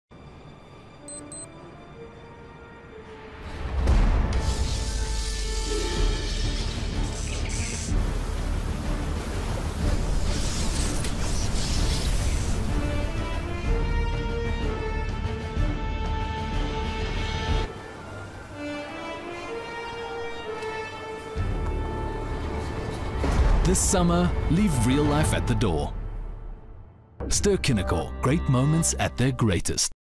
Ster-Kinekor - Leave Yourself at The Door
Agency  : AHEAD MARKETING SERVICES
Campaign : Leave Yourself at The Door
Advertiser : Ster-Kinekor
Brand  : Ster-Kinekor
Length  : 30 Seconds
Posted   : Dec 8, 2023

In this Movie Ad we’ll walk you through:
Ster-Kinekor's campaign, "Leave Yourself at The Door," immerses viewers in a cinematic experience that goes beyond traditional storytelling.

TIMESTAMPS :
00:00 Ster-Kinekor - Leave Yourself at The Door
Plot :
Begins with a slow pan up to the Ster-Kinekor branded theater door, focusing on the doorman checking his watch. As the door opens, audiences dressed as movie characters exit, emphasizing the immersive nature of cinema. The spotlight then shifts to a middle-aged couple, who, after experiencing the magic of Ster-Kinekor, transform into Aquaman and Mera, complete with CGI effects like a majestic wave. The transformative journey continues as they transition back to their real-world selves, showcasing the revitalizing impact of the Ster-Kinekor experience. The ad concludes with a black screen, featuring the Ster-Kinekor logo and the tagline: "Ster-Kinekor. Great moments at their greatest."